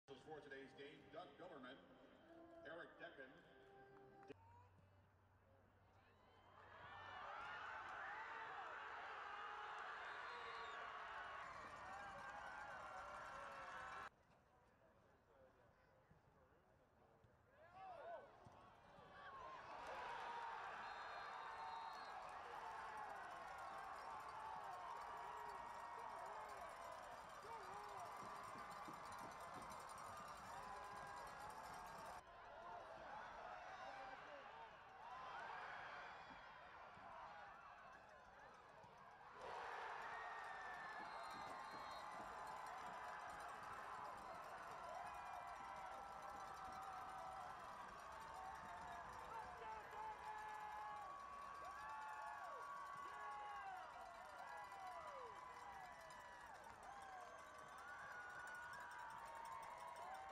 State champs! Howland Boys Soccer snaps Tippecanoe's 47-game win streak
Howland won the first soccer state title in program history with a 4-0 win over Tipp City Tippecanoe in the Division II Boys Soccer State Final.